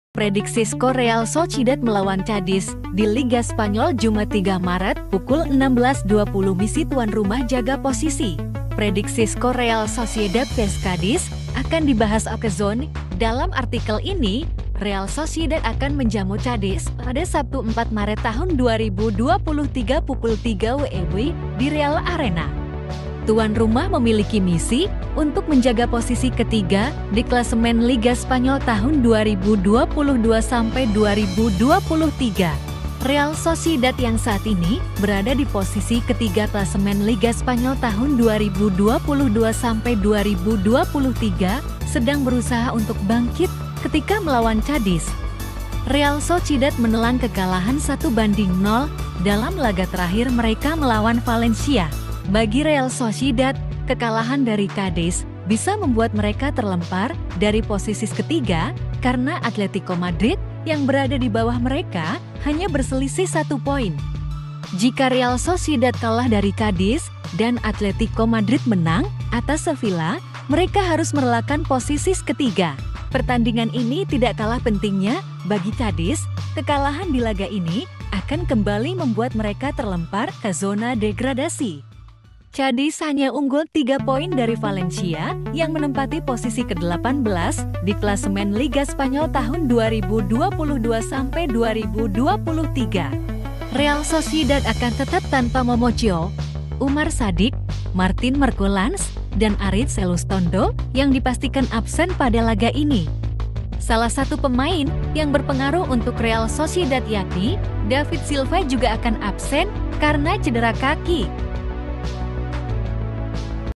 liga spanyol... real sociedad vs cadis super foodball liga spanyol...
PREDIKSI skor Real Sociedad vs Cadiz akan dibahas Okezone dalam artikel ini. Real Sociedad akan menjamu Cadiz pada Sabtu 4 Maret 2023 pukul 03.00 WIB di Reale Arena, tuan rumah memiliki misi untuk menjaga posisi ketiga di klasemen Liga Spanyol 2022-23.

Real Sociedad yang saat ini berada di posisi ketiga klasemen Liga Spanyol 2022-23 sedang berusaha untuk bangkit ketika melawan Cadiz. Real Sociedad menelan kekalahan 1-0 dalam laga terakhir mereka melawan Valencia.

Selebrasi pemain Real Sociedad ketika mencetak gol (FOTO: REUTERS)

Sementara itu, Cadiz ingin melanjutkan momentum bagus mereka setelah meraih kemenangan penting untuk membantu perjuangan mereka menghindari jurang degradsi. Cadiz menang tipis 1-0 dari Rayo Vallecano pada laga terkhir mereka.

Bagi Real Sociedad, kekalahan dari Cadiz bisa membuat mereka terlempar dari posisis ketiga karena Atletico Madrid yang berada di bawah mereka hanya berselisih satu poin. Jika Real Sociedad kalah dari Cadiz dan Atletico Madrid menang atas Sevilla, mereka harus merelakan posisis ketiga ke anak asuh Diego Simeone.

Pertandingan ini tidak kalah pentingnya bagi Cadiz, kekalahan di laga ini akan kembali membuat mereka terlempar ke zona degradasi. Cadiz hanya unggul tiga poin dari Valencia yang menempati posisi ke-18 di klasemen Liga Spanyol 2022-23.

BACA JUGA:

￼Tahan Imbang Real Madrid, Pelatih Real Sociedad Girang Bukan Main: Ini Kesuksesan Besar!

Real Sociedad akan tetap tanpa Momo Cho, Umar Sadiq, Martin Merquelanz, dan Aritz Elustondo yang dipastikan absen pada laga ini. Salah satu pemain yang berpengaruh untuk Real Sociedad yakni David Silva juga akan absen karena cedera kaki.